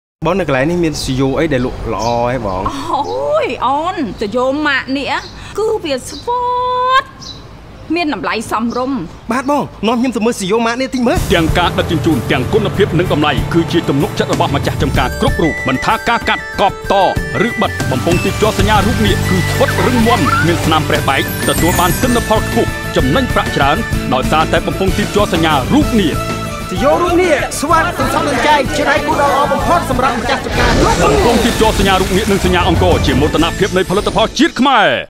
Dragon Pipe TVC
My Pipe My Farm

We spent 2 days for production of Dragon Pipes at Pilin Province with nuclear teamwork because we need a lot of cast and farmer. You know? the last day of shooting is the sad day because of raining. we need to change the scene immediately, and this is the reason which there is some scene unfollow the storyboard below. We have to shoot under the rain until it was done perfectly. Such a stupid thing the god challenge us...lol

Client: BHL Angkor
Product: Dragon Pipe
Producer: Heap Bunden
Director: Sara Vorn
Assist. 1st Sopheak | 2nd Malis
DOP: David Rivera
Editor: Vichet
Voice Over: Pin Sonang
Post: Legend Tiger
Length: 35 Second
Quality: 4K
Camera: SonyF55
Released: 05-June-2017

Cast: Preap Sovat | Ming Krot | Sim Sophorn | Chreung Sreyhouy | Vireak | Vitou